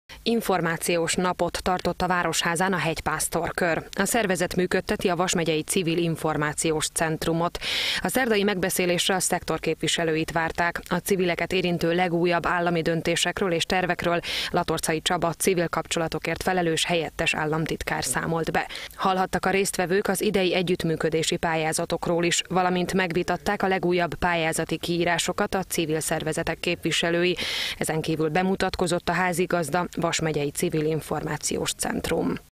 Információs napot tartott a városházán a Hegypásztor Kör
A szervezet működteti a Vas Megyei Civil Információs Centrumot.